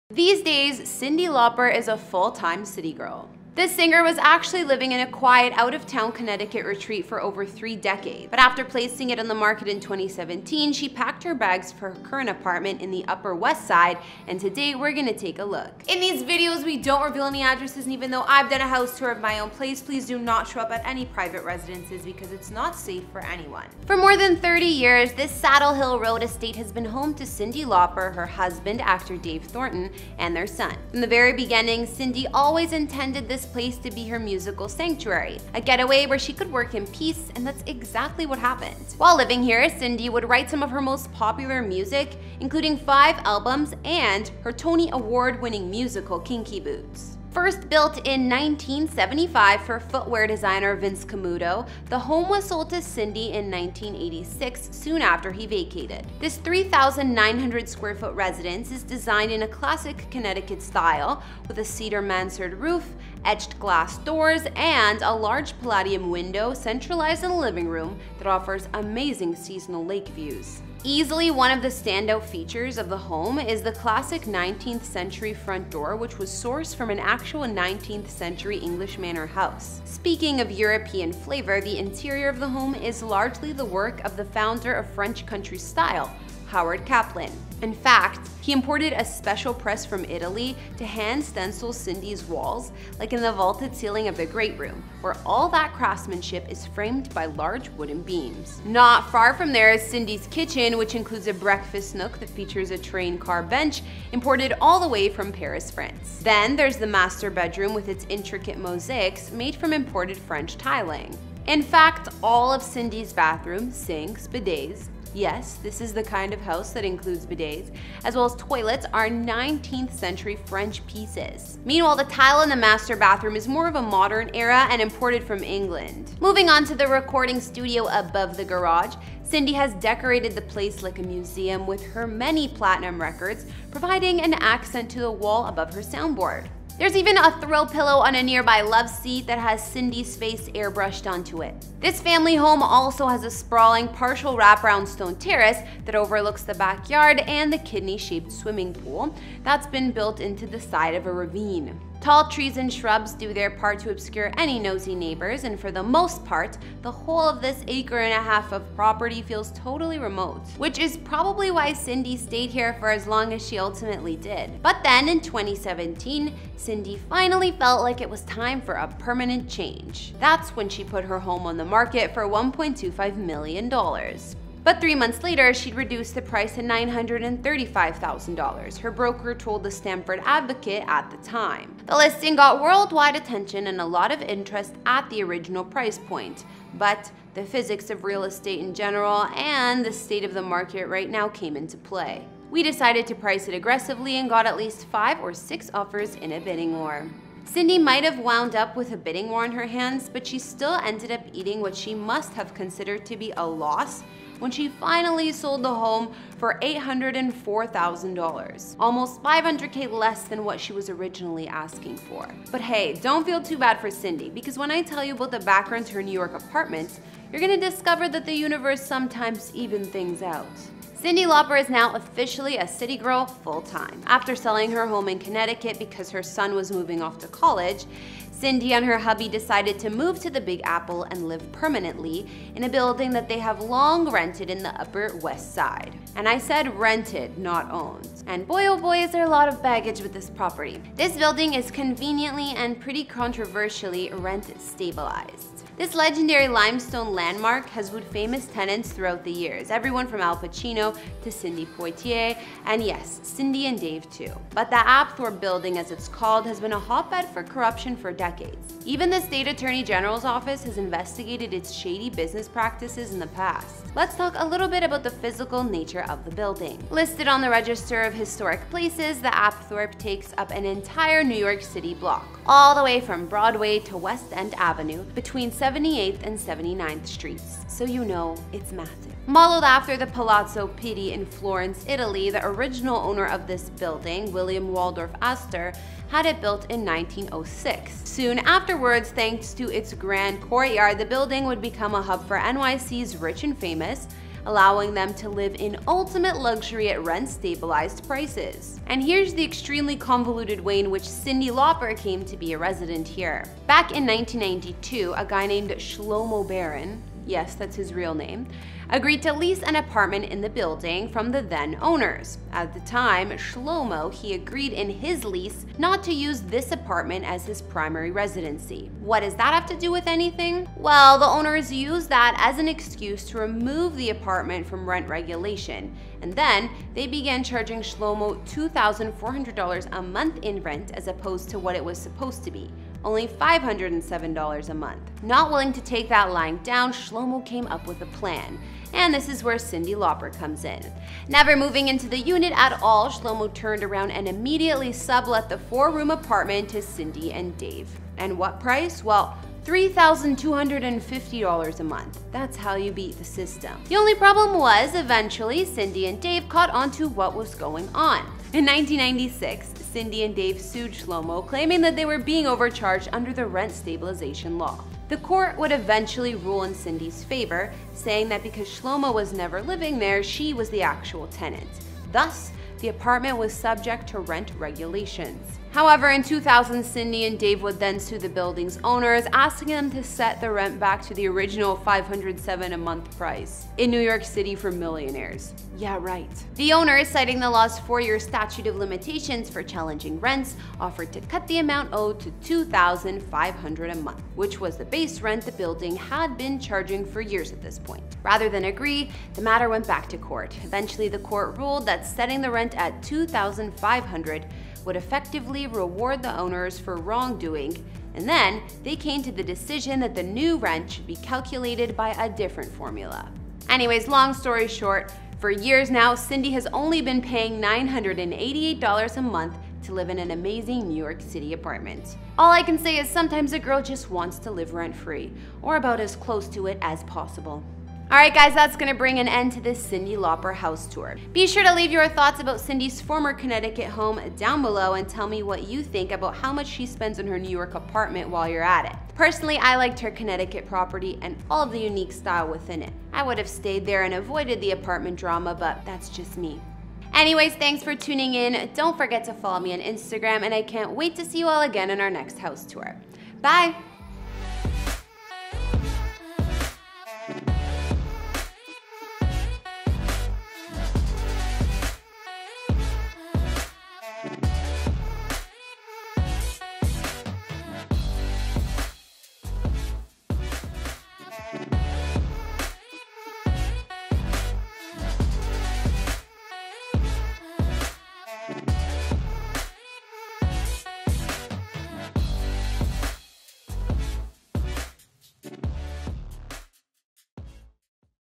Cyndi Lauper | House Tour | Luxurious $2 Million New York Mansion & More
These days, Cyndi Lauper is a full-time city girl. The singer was actually living in a quiet, out-of-town Connecticut retreat for over 3 decades – but after placing it on the market in 2017 – she packed her bags for her current apartment in the Upper West Side, and today we’re going to take a look.